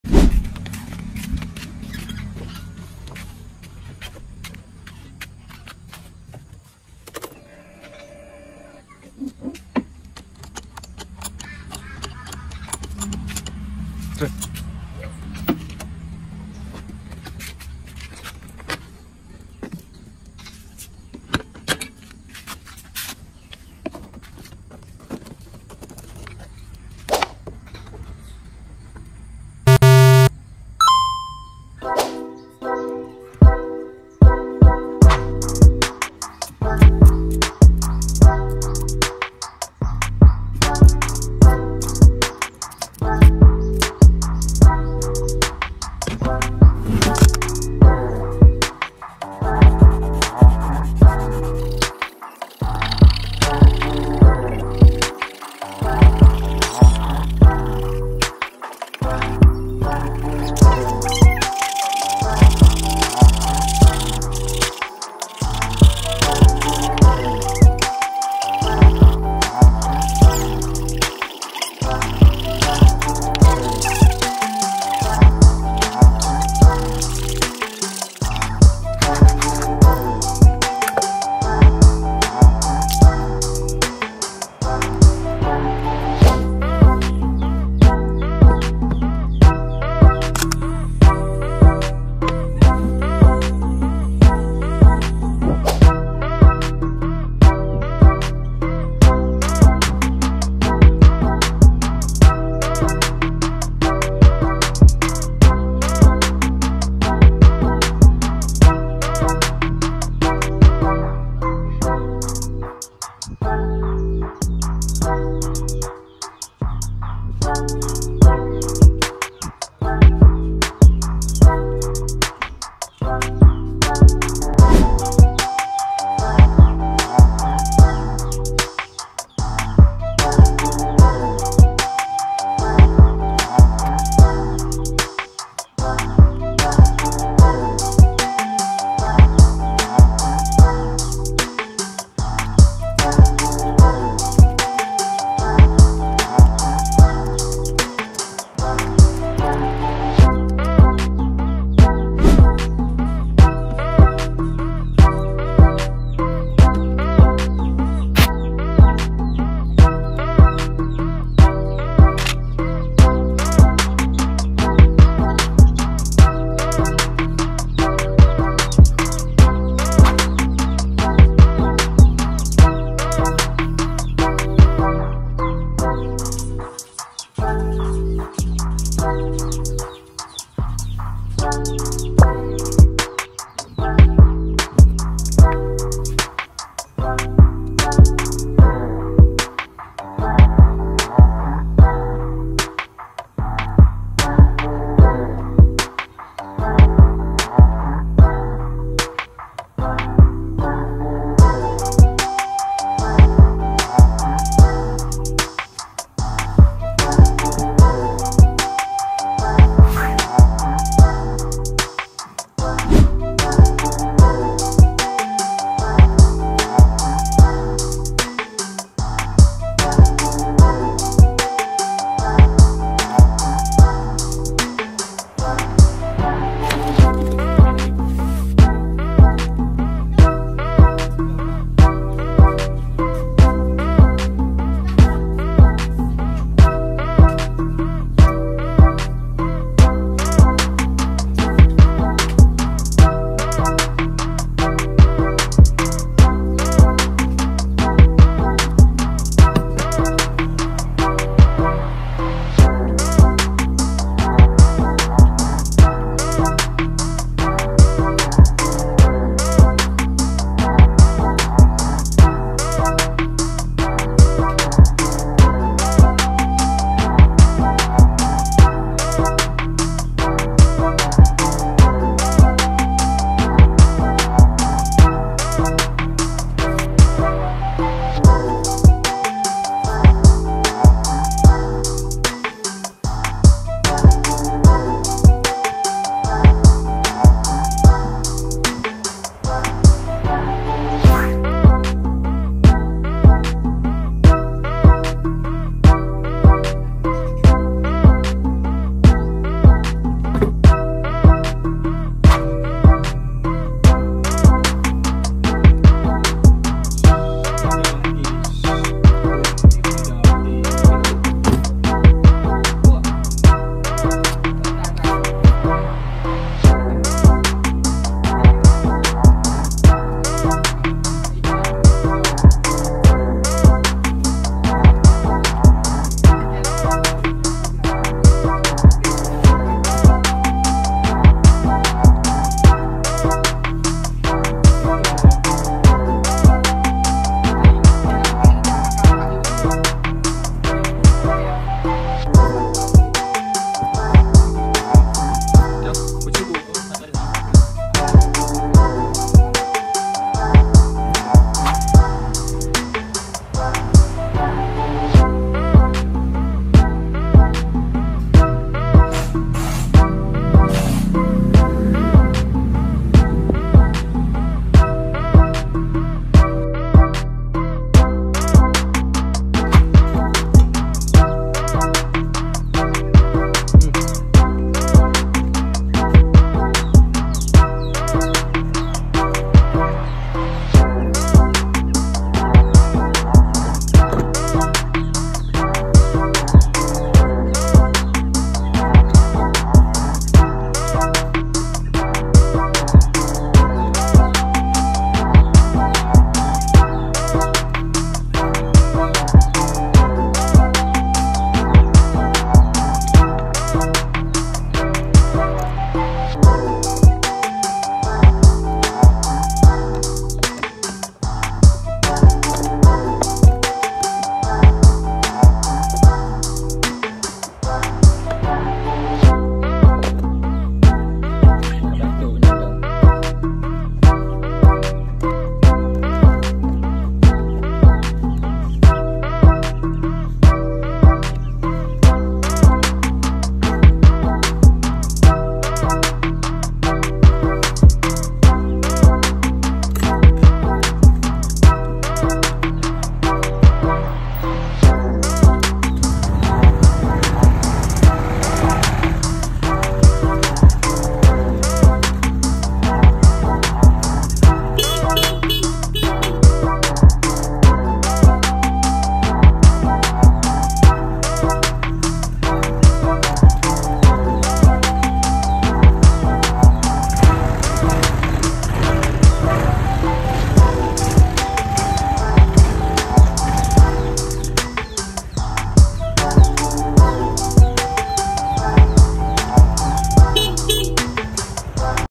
Technique for repairing a dead 12v battery with the help of solder
Here's how to renew a broken battery or how to revive a dead battery with the techniques I teach. how to repair a dead battery or how to renew a 12 volt battery, you can listen to some of the methods I teach. How to revive a dead battery or how to solve a dead battery, you can solder some elements in it that make the battery dead.